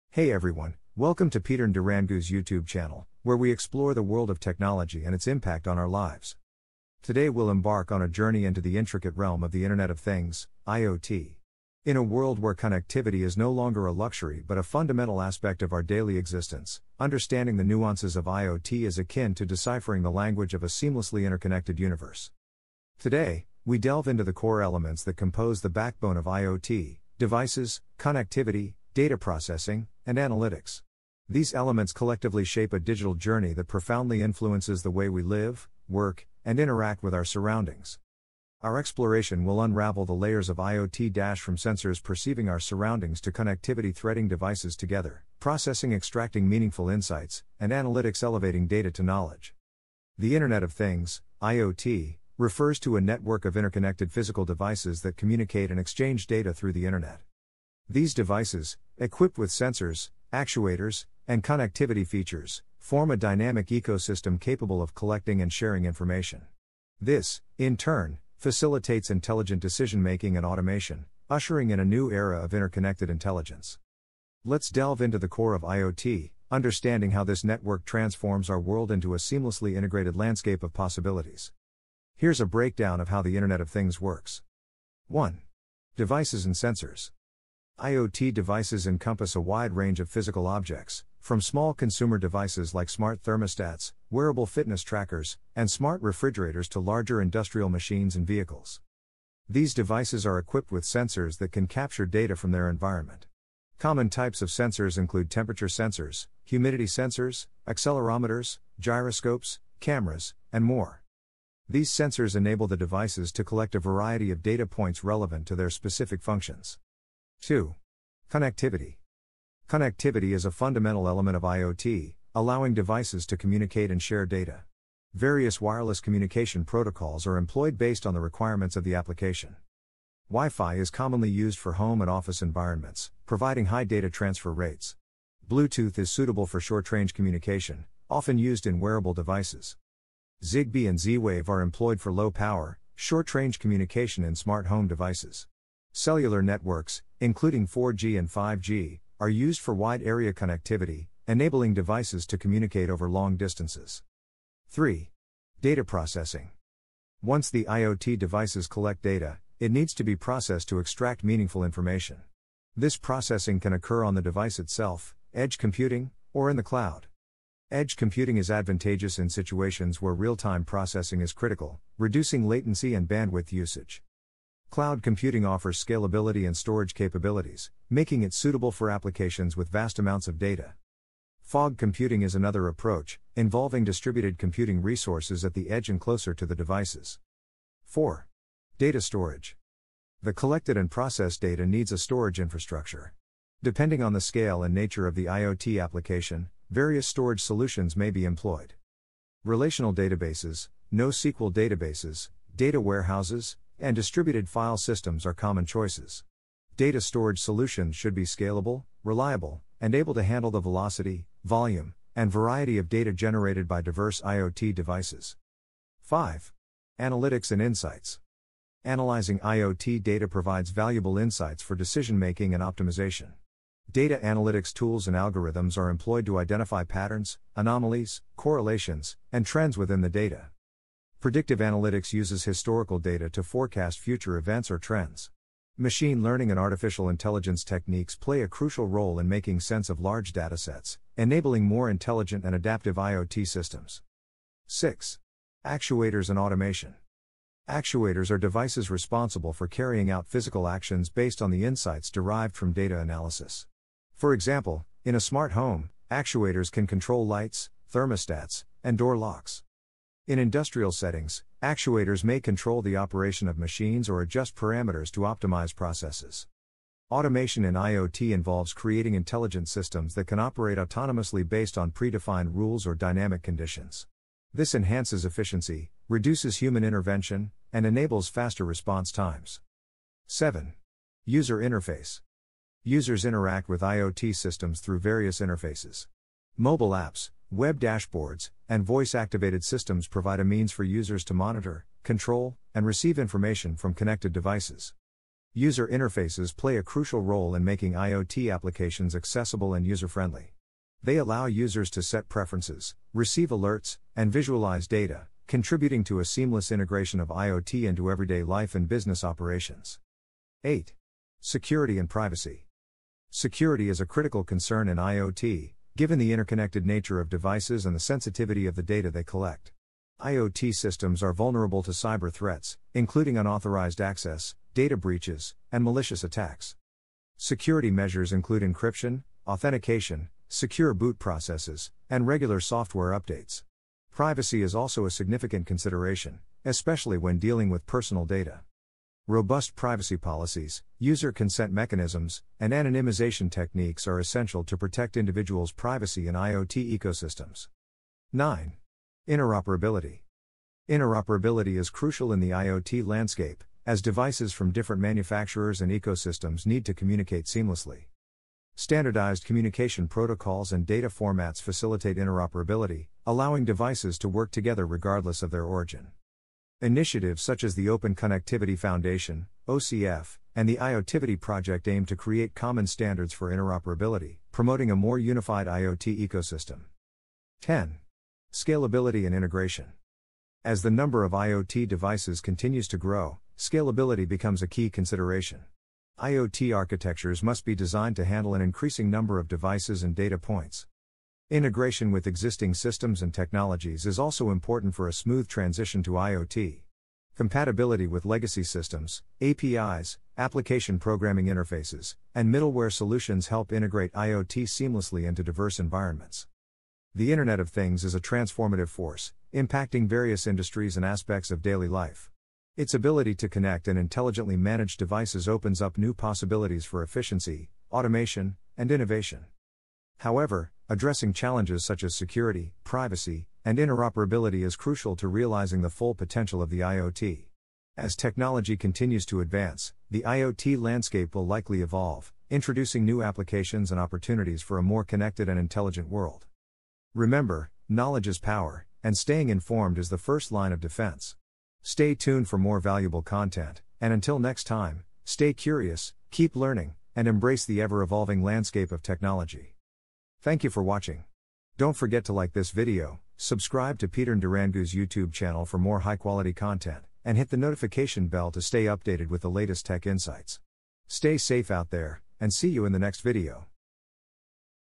Unleashing the Future: Navigating the Internet of Things (IoT) Landscape
Embark on a captivating journey into the heart of the Internet of Things (IoT), where the ordinary transforms into the extraordinary. Join us as we demystify the complexities of IoT, exploring its definition and delving into the dynamic world of interconnected devices. From sensors to intelligent decision-making, discover how the IoT landscape is shaping the future, creating a realm of possibilities. Unleash the power of connectivity and automation – your gateway to a smarter, more interconnected world awaits!